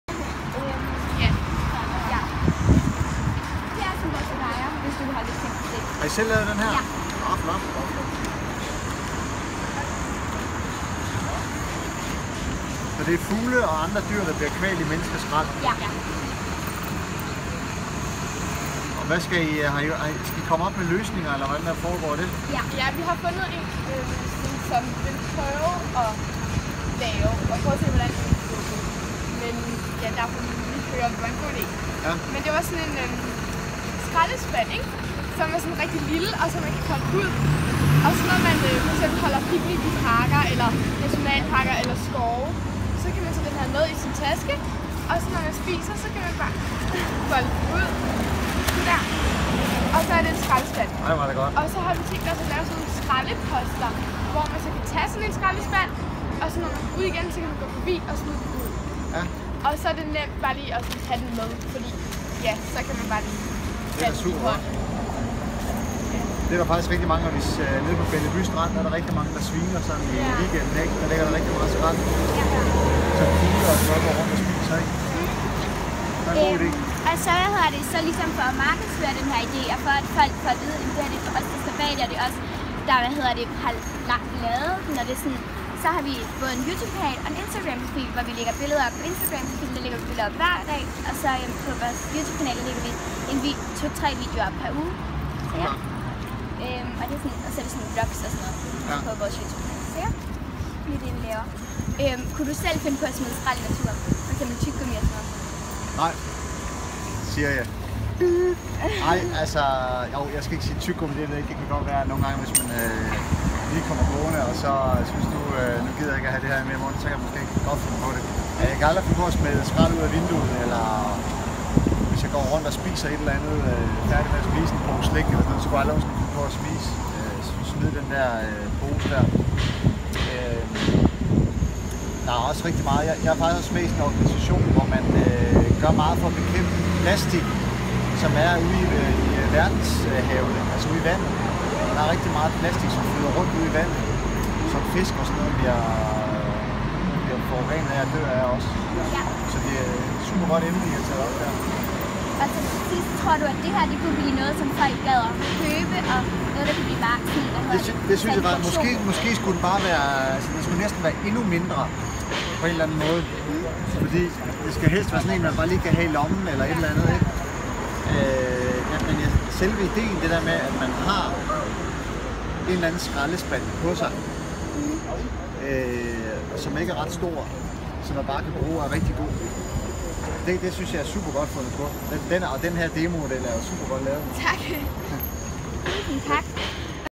Interweiw med PETER MØLLER!?
Vi snakker/spørge Peter Møller om nogle spørgsmål og snakker lidt med ham, se med!

Håber i kan lide den.😊